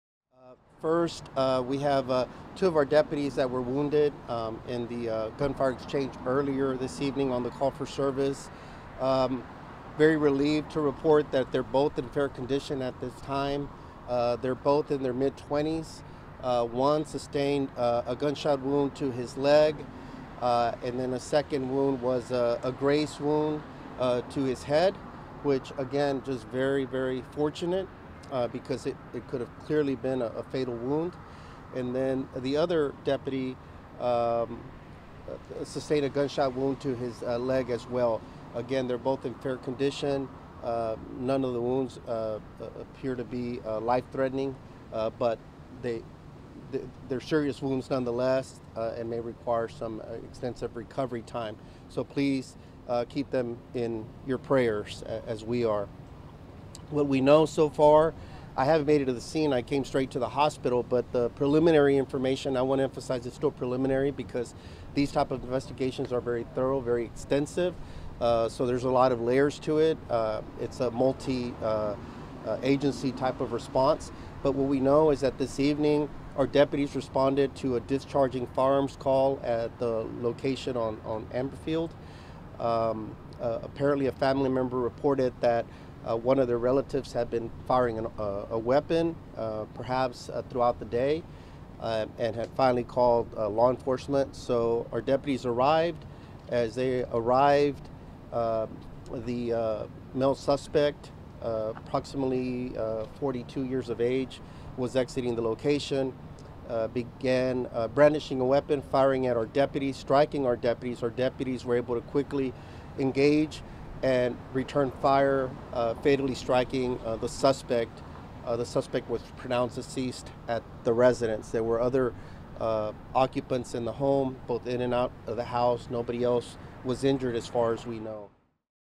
Sheriff Ed Gonzalez gives update from hospital after 2 deputies were shot
Authorities said deputies responded to reports of shots being fired, and when they arrived, the suspect opened fire on them.